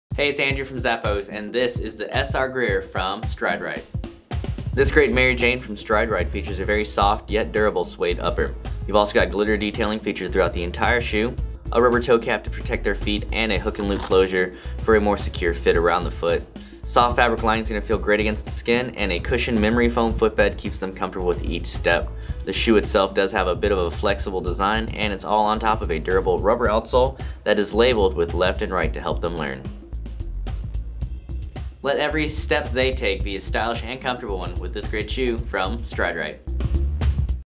Stride Rite SR Greer (Toddler) SKU: 9273877
stride rite&reg; Size Chart
Cool cutouts and fresh color are the free-spirited fall uniform. She'll play all day in the Stride Rite&reg; SR Greer Mary Jane.
Premium leather upper with glitter detail.
Adjustable hook-and-loop strap closure.
Signature memory foam footbeds for enhanced comfort.
Seamless construction that cradles the foot's natural shape.
Twill linings with antimicrobial treatment to reduce odor.
Padded heel collar for all-day comfort.
'Left' and 'Right' sole markings for easy identification.
Flexible rounded soles to encourage natural movement and improve balance.
Traction-enhanced rubber outsoles help with grip and grounding.
Imported.

Product measurements were taken using size 8.5 Toddler, width M. Please note that measurements may vary by size.